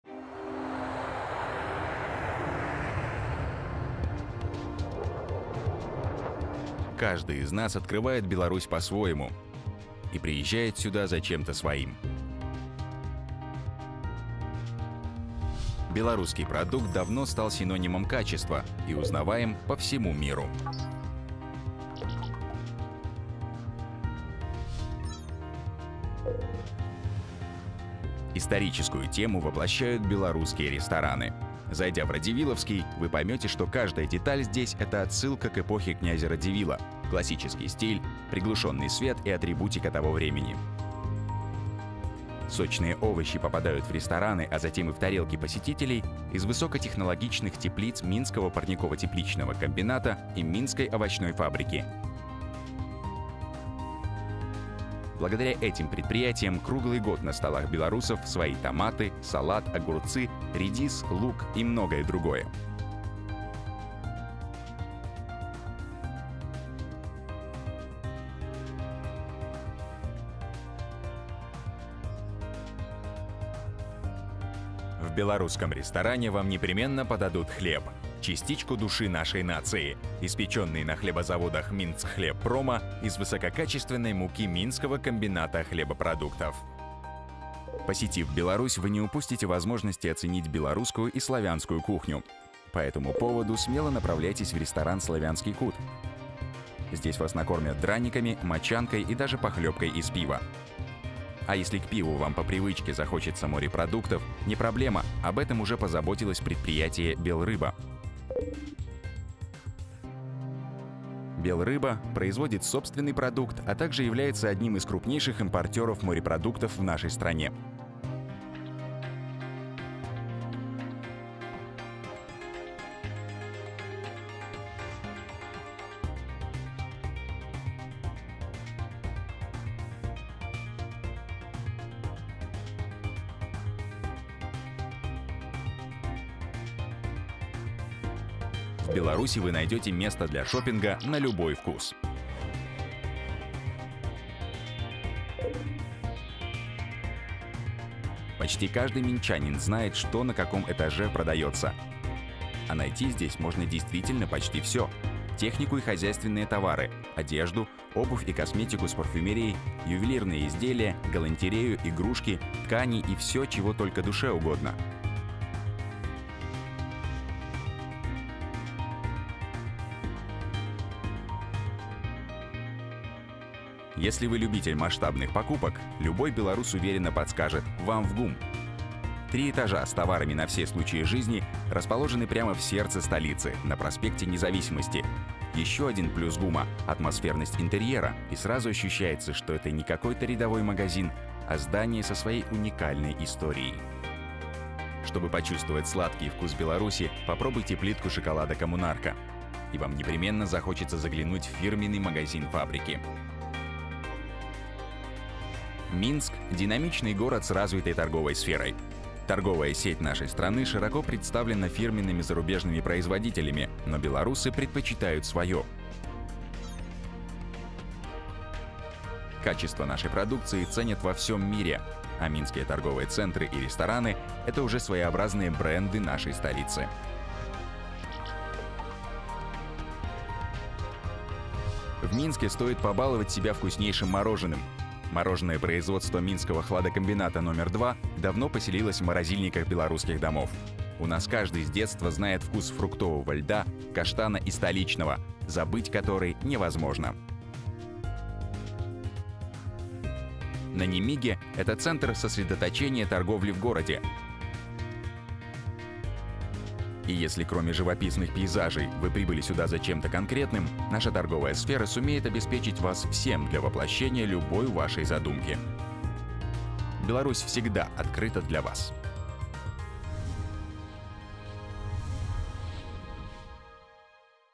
Полная версия.Реклама продовольственной сферы Беларуси ГУПР ( "Коммунарка", "Гум", "Цум" )
Nonstop Media - полный цикл производства
Генеральный продюсер: Сергей Жданович
Режиссер-постановщик: Татьяна Хацкевич
Оператор-постановщик: Анна Беленикина
Актеры: Руслан Чернецкий и Олеся Грибок
Монтаж и цветокоррекция: Сергей Казаков и Андрей Голдырев
Графика: Павел Лапковский, Евгений Врублевский (fuzzme production)